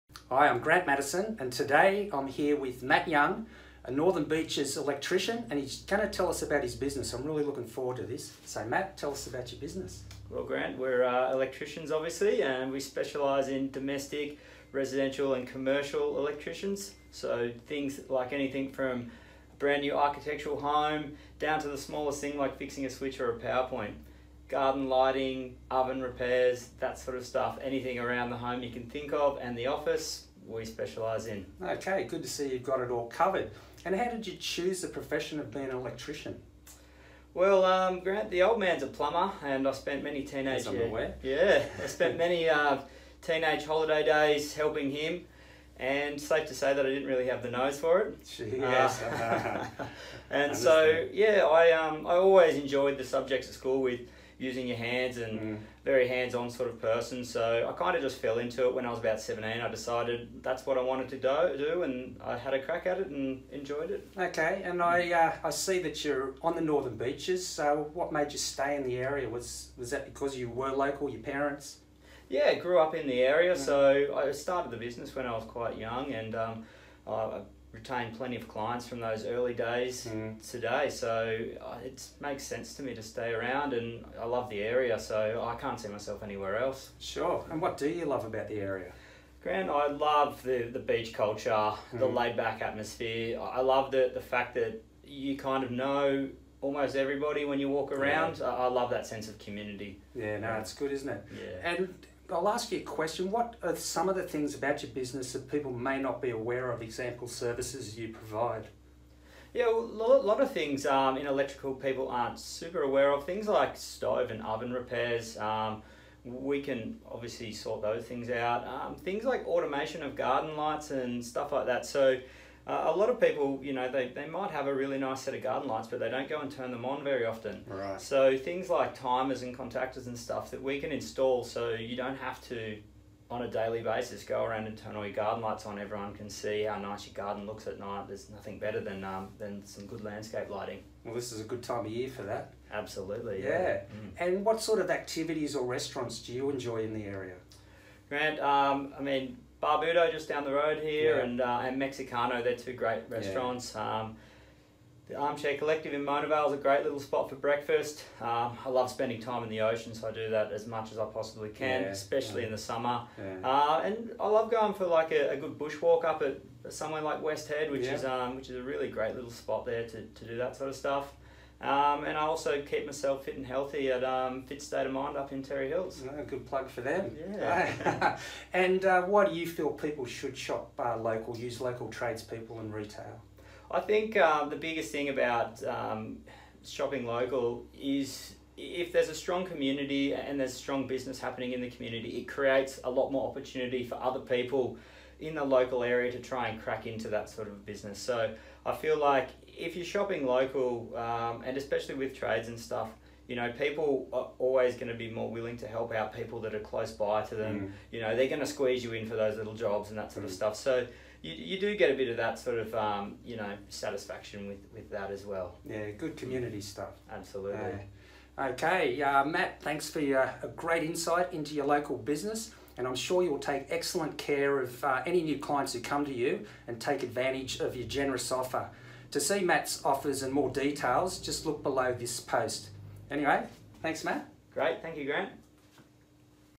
Matt Young Electrical in 2101, meet the Owner Matt Young interviewed by Grant Matterson
I am here today with Matt Young and Matt is a local Electrical Tradesman and I am looking forward to hearing about his local business

• So, Matt Tell us about your business?
• What made you open your business in this location?
• Why did you choose the profession as a Electrician?
• What is something most people don't know about your business? E.g. the services you provide.
• What do you love about the local community?
• What are your favourite restaurants or activities in the area?
• Why do you think it is important for people to shop local and use local businesses?
Thank you, Matt, that was a great insight into your local   business, and I am sure you will take excellent care of any new clients who take advantage of your generous offer.

Grant Matterson LJ Hooker Narrabeen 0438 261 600
Matt Young at Matt Young Electrical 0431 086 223